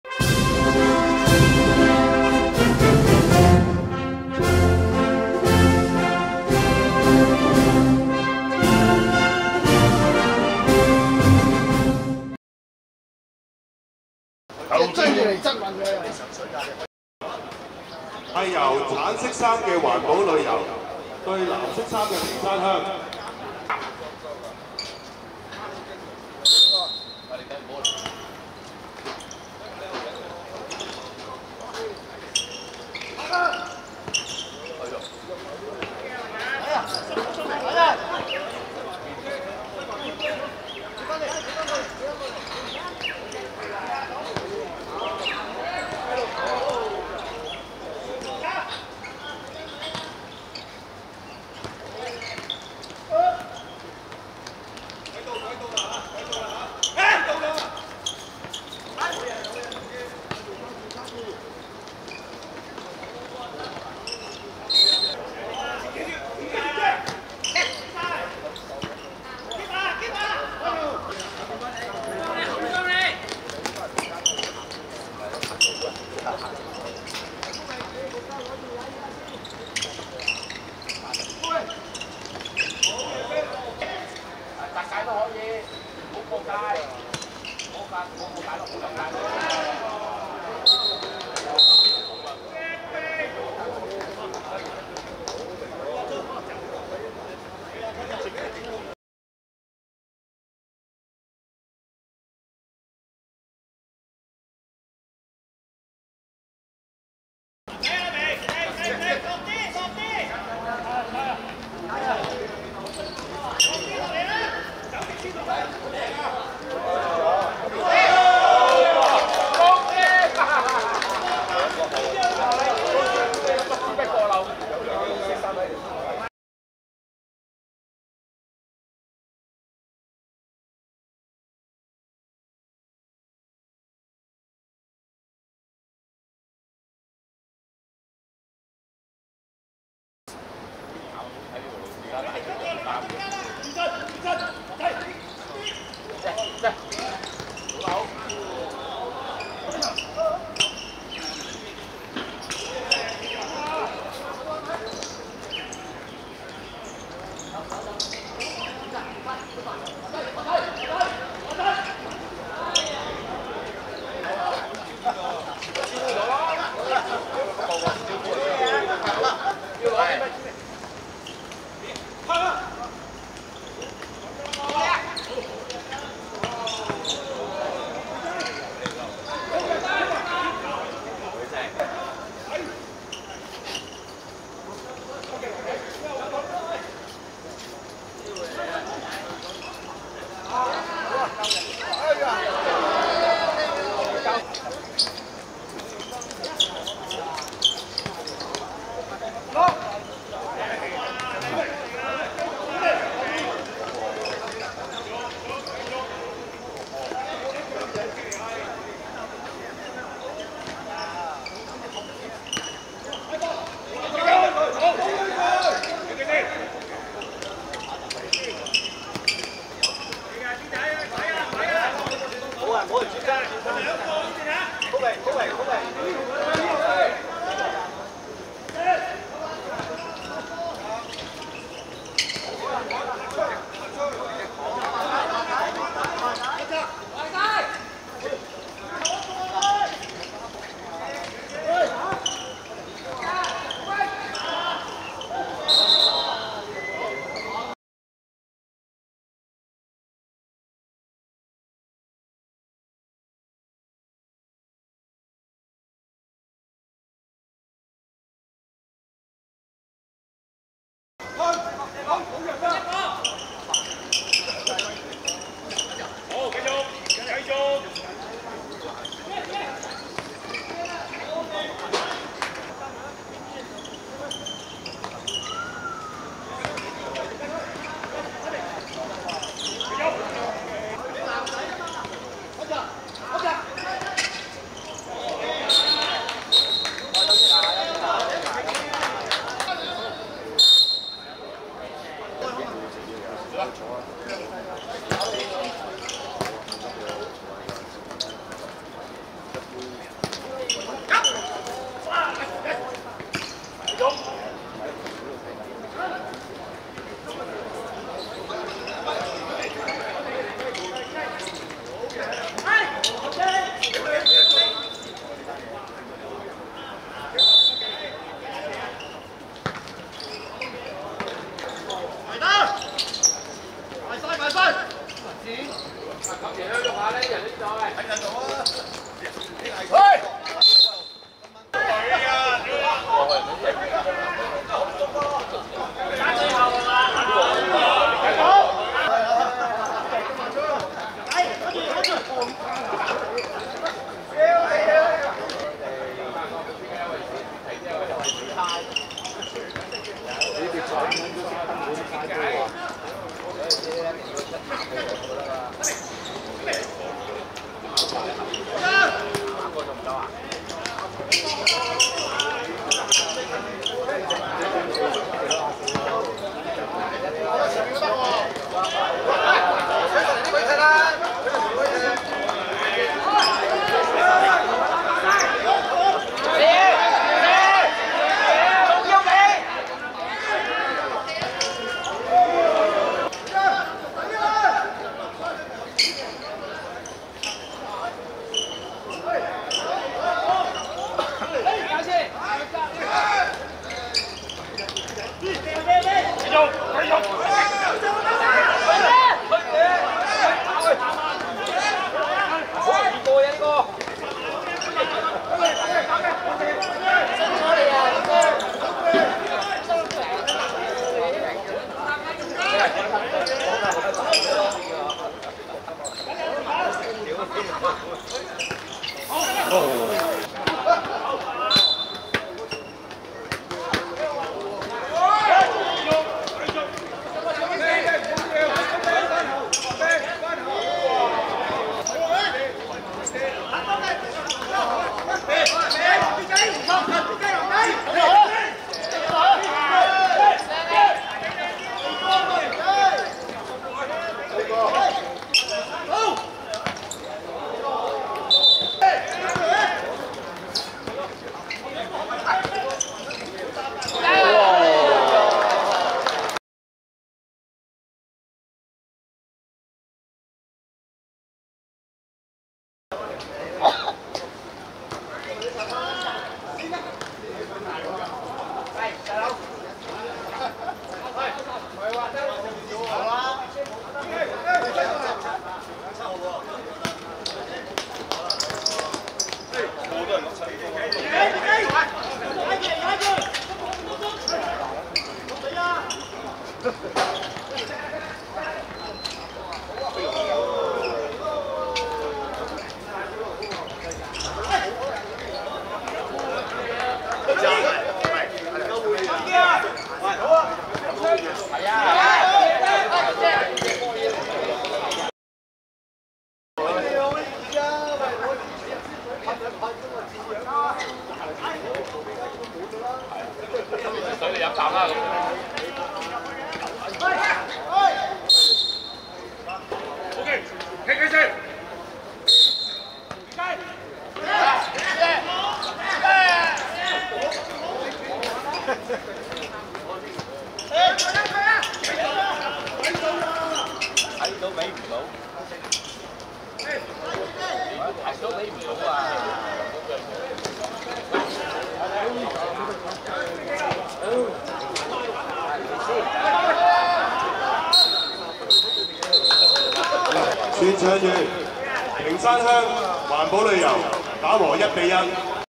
屏山鄉vs環保旅遊(2023.9.14.元朗屯門七鄉國慶盃七人足球邀請賽~60歳組)精華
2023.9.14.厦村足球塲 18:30
屏山鄉(藍) 1 : 1 環保旅遊(橙)